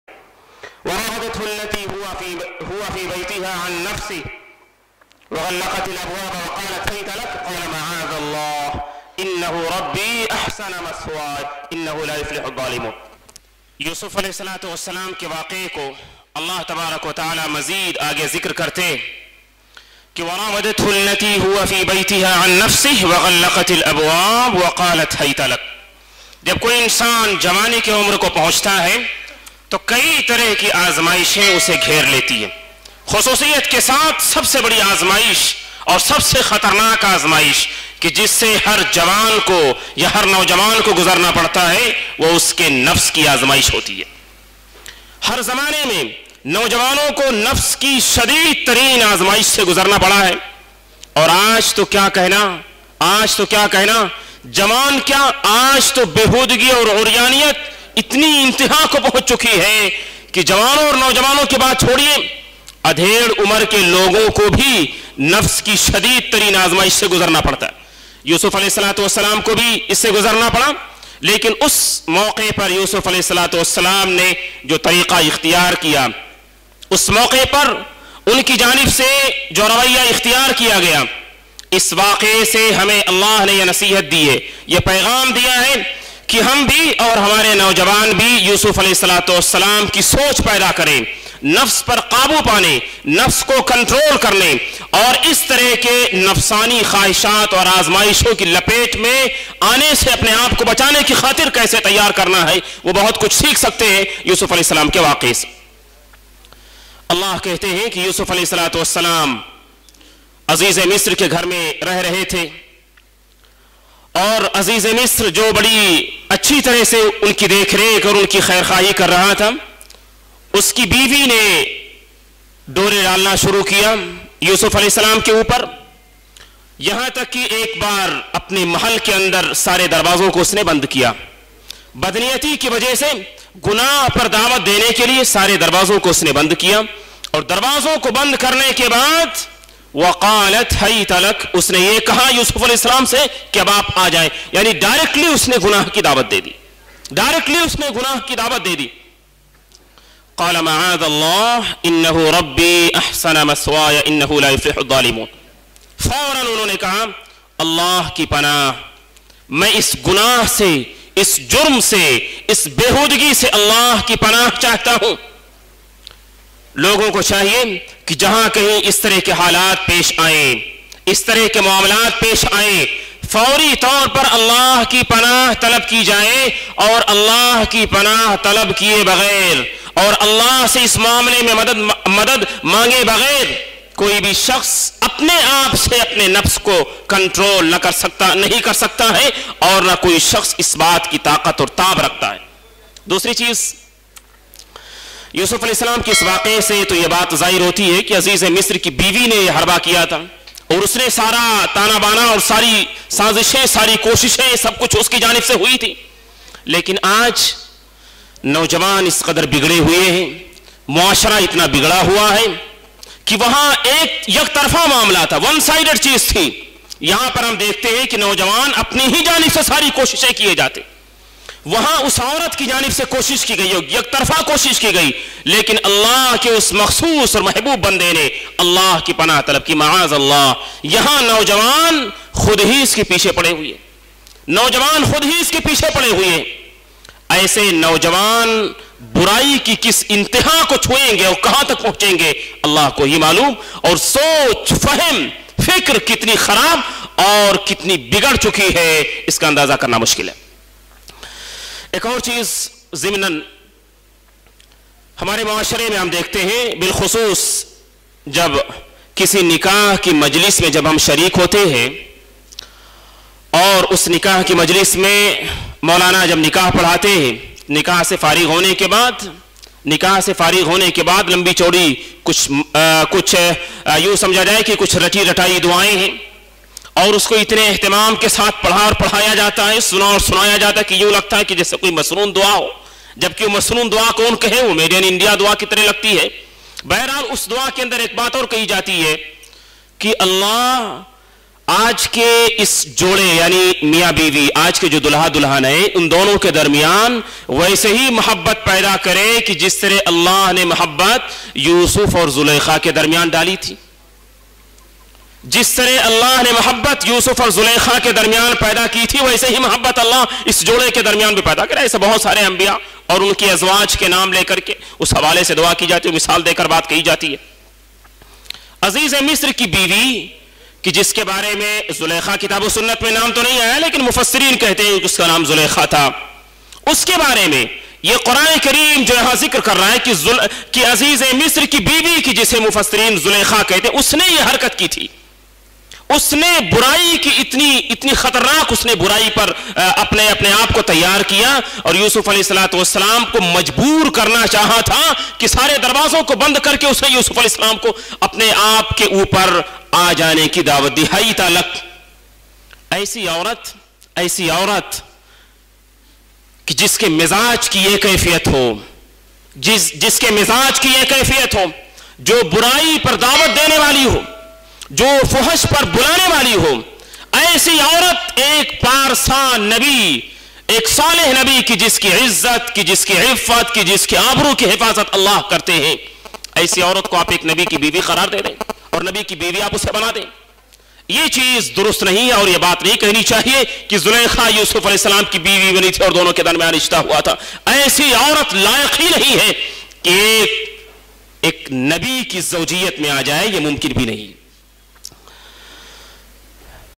Nafs ki azmaish | Shaikh Habeeb ur Rahman Salafi
This video clips is taken from JAWAANI EK AZEEM NIAMAT by Shaikh Habeeb ur Rahman Salafi at Itaate Rasool Conference